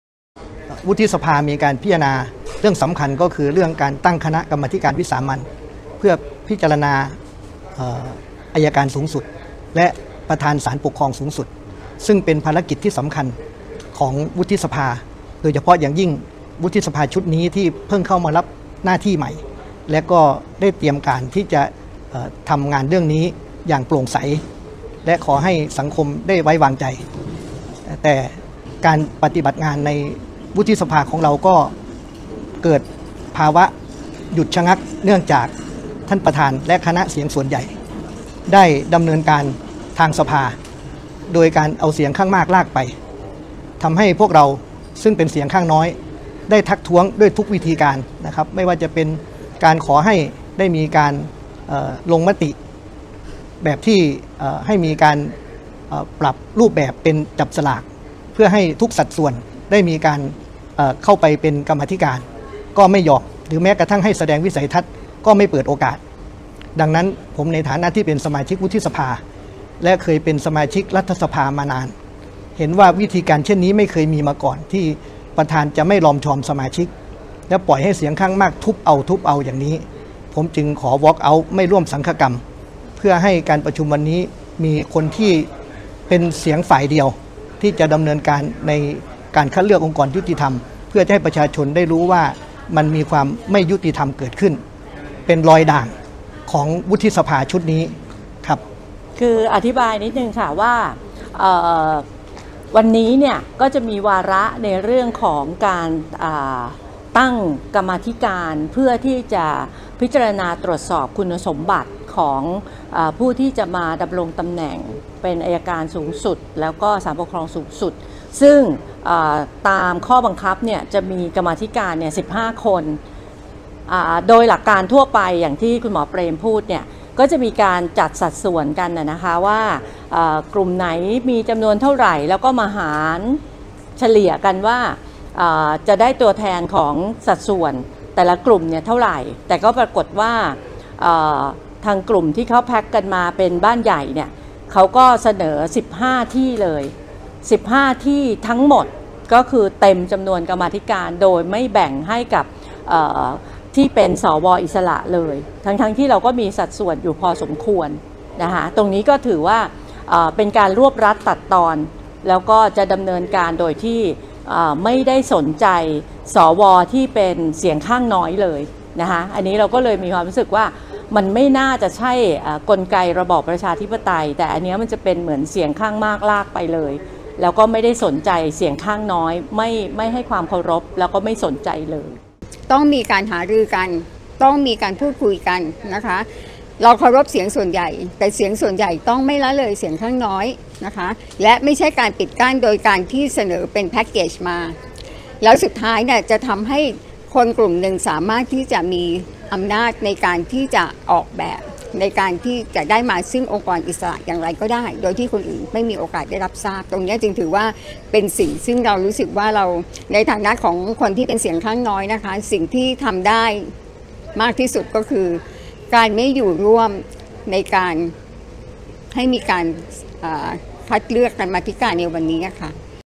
สว.พันธุ์ใหม่-อิสระ แจงใช้พวกมากลากไปจึงเมินสังฆกรรม จี้ปธ.ทบทวนท่าทีอย่าซ้ำรอยเผด็จการวุฒิฯ
"สว.พันธุ์ใหม่-อิสระ" แจงเหตุไม่ร่วมสังฆกรรม ตั้ง เหตุใช้พวกมากลากไป กมธ.สอบประวัติ อสส. บี้ ปธ.ทบทวนท่าที หากยังฝืนทำอาจซ้ำรอยเผด็จการวุฒิสภา อ้าง แค่แสดงสันติวิธี พรุ่งนี้ยังมาทำงานต่อ